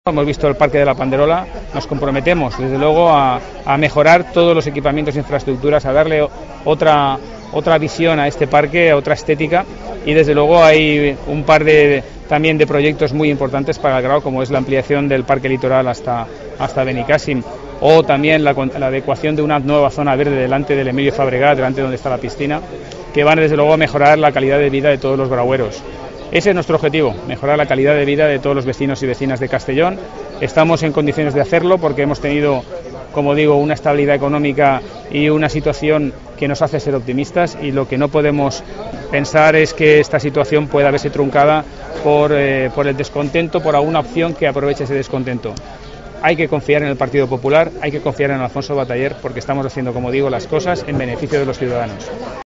Moliner arenga a los suyos: "Es el momento de los valientes"
"Es el momento de los valientes, de garantizar nuestro futuro y de poner Castellón en mayúsculas. La identidad con nuestro territorio y nuestra provincia es mucho más que una mera declaración de intenciones, es el ejercicio institucional de priorizar las oportunidades de nuestra tierra y de nuestros vecinos, es apostar por el talento de las personas de Castellón, es defender nuestras tradiciones y lo que somos como símbolo común llamado Provincia de Castellón". Así ha explicado el presidente provincial del Partido Popular de Castellón, Javier Moliner, la prioridad que ha establecido el PPCS en la defensa de la identidad e intereses de la provincia de Castellón como eje vertebrador de su programa de Gobierno para las próximas elecciones, el único redactado en clave provincial.